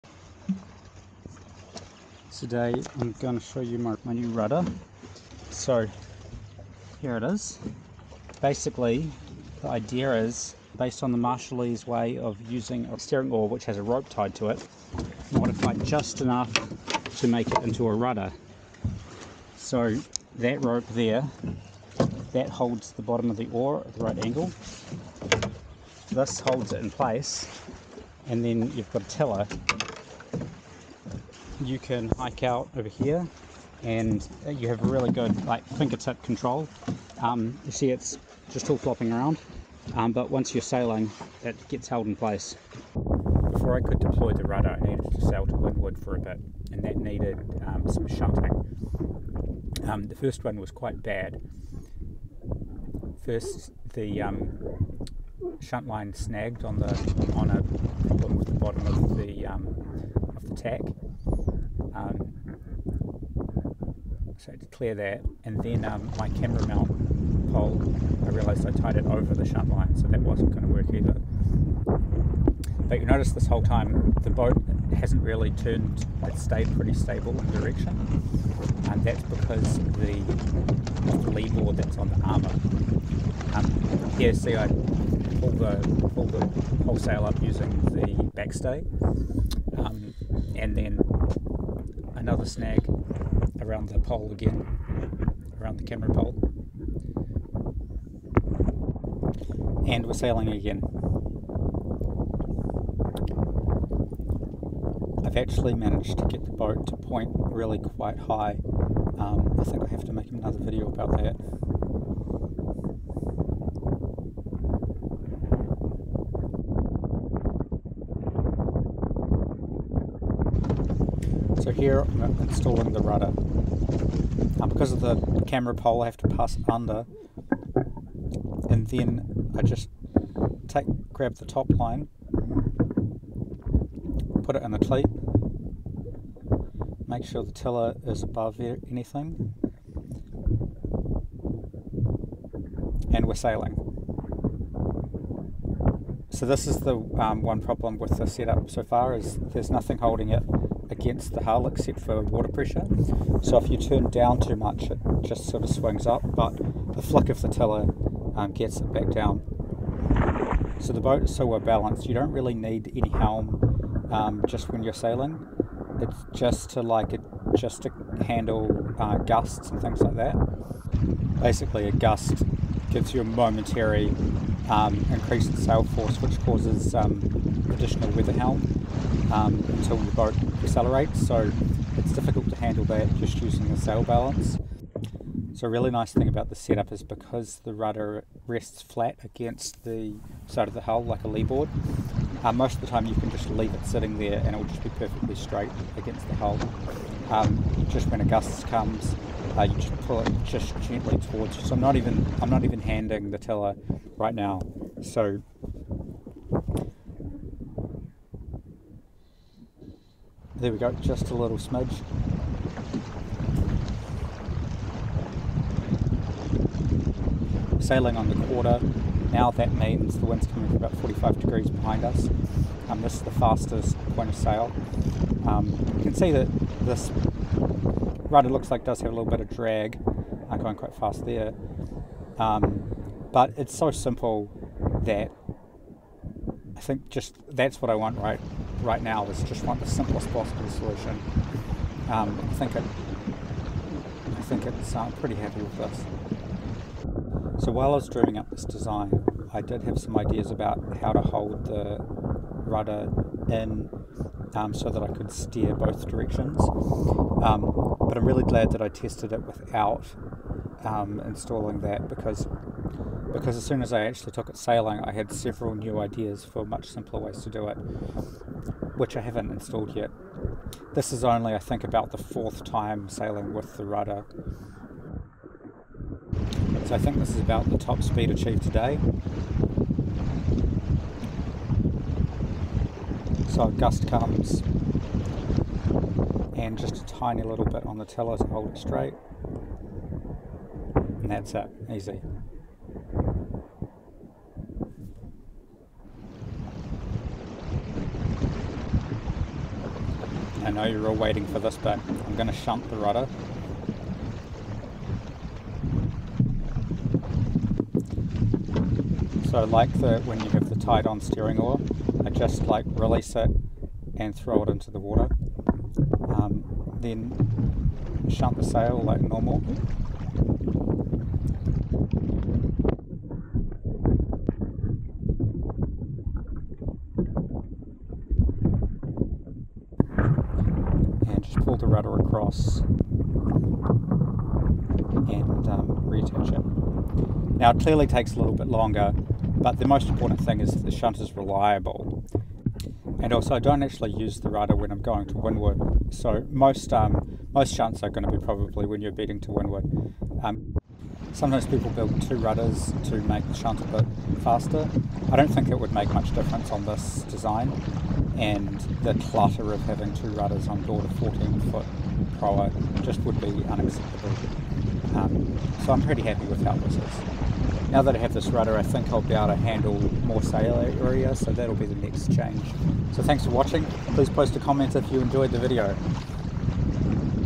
A Simple Proa Rudder - Waimea Proa E5
One of the first thing you learn about proas is that it's hard to design a rudder for them. Well, here is my design. It's inspired by the way a steering oar is used on the marshall island (and others). An oar is tied to a line, the other end attached amidships. The oar is then held to the lee side of the main hull.
In my version, I've got another line at the top and a cleat to hold it in place, and have a tiller and extention on the oar so that it may operated single handed from the platform - while trimming the sheet and providing righting moment!